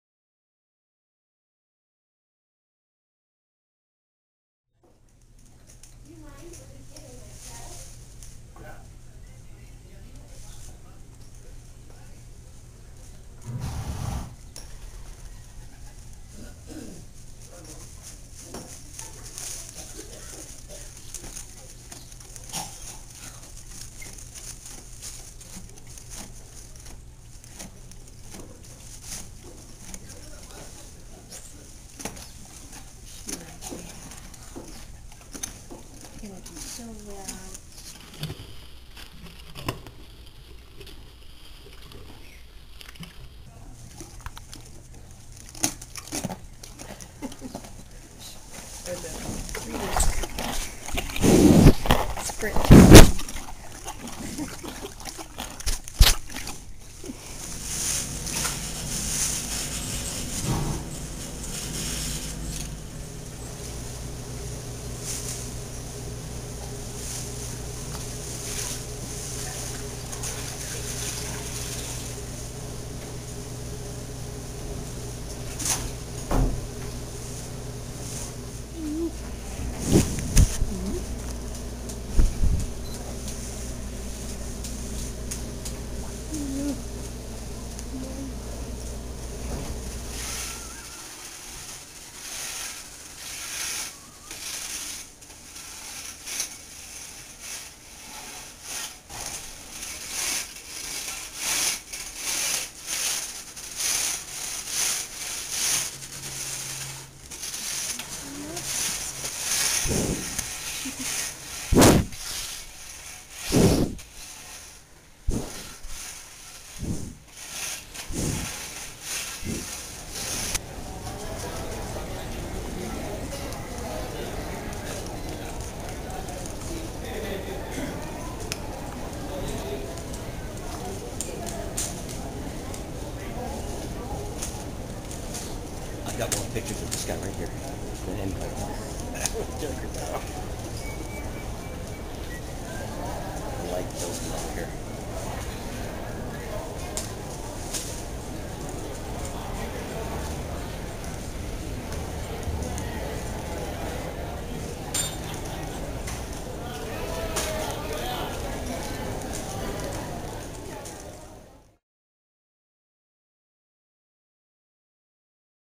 California-bred Stars at Santa Anita - Oct. 29, 2011 (Amazombie, California Flag, Caracortado)
California-bred stars and Breeders' Cup World Championships contenders Amazombie, California Flag and Caracortado in their stalls at Santa Anita Park racetrack in Arcadia, Calif., USA. Caracortado is also filmed schooling in the paddock in the afternoon. Thanks to my friend Edie, seen feeding treats, for her help. (Oct. 29, 2011)